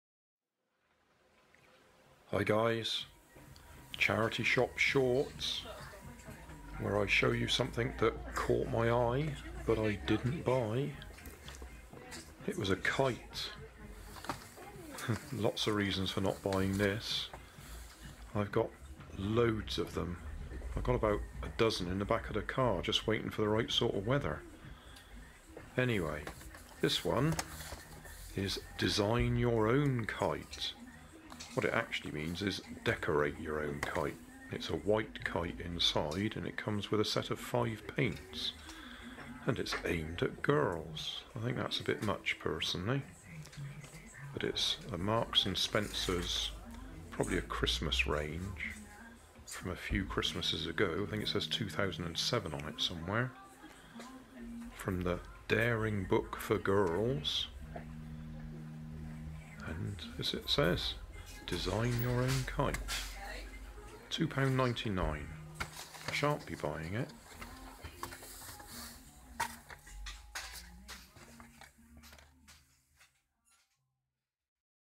Charity Shop Shorts - Design your own Kite
Charity Shop Shorts - Design your own Kite. One of the purposes of these charity shop short series is to demonstrate the randomness of the items that turn up in these shops. I cannot afford to buy everything that catches my eye.

Sold by Marks & Spencers in 2007 and bassed on the book "The Daring Book for Girls" - Design your own kite. Looks like a white kite and a set of paints.

Camera LG Spirit mobile phone